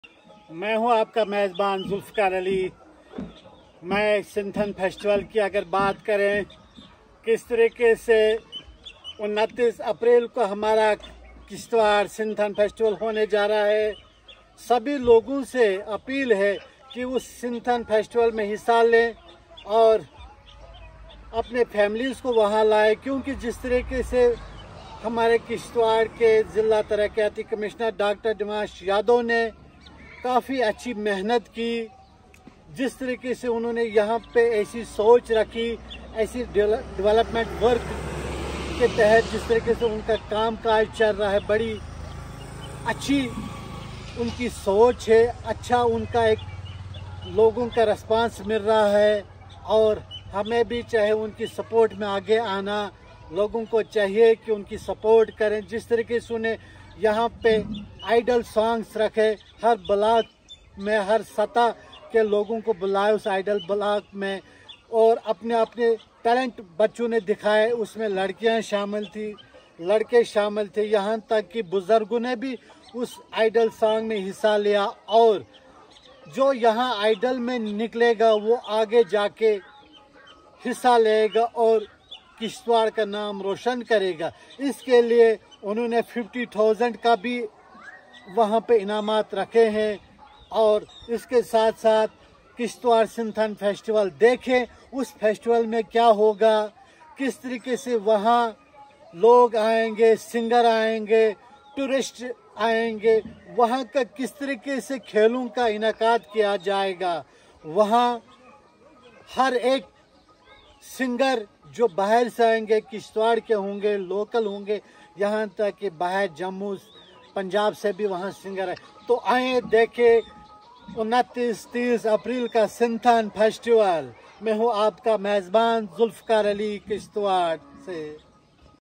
Sinthan festival scheduled on 29th-30th April 2023 at Sinthan Maidan
Sinthan festival scheduled on 29th-30th April 2023 at Sinthan Maidan & Sinthan Top, District Kishtwar.